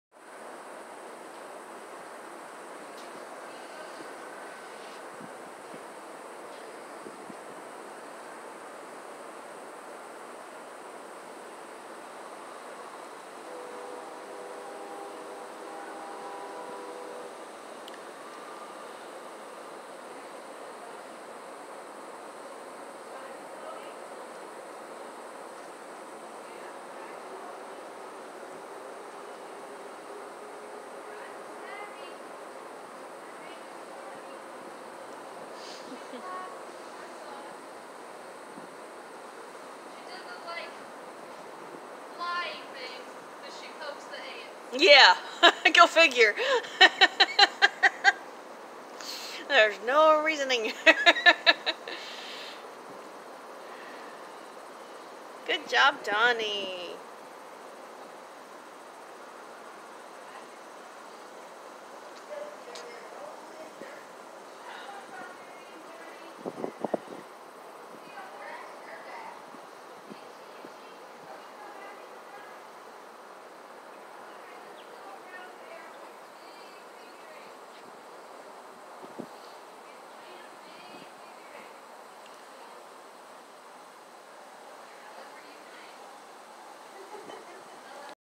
Our Daughter, Doni, at Stable Spirit.
Stable Spirit is a 501(c)(3) non-profit organization that teaches children and adults with disabilities like Autism to groom and ride horses.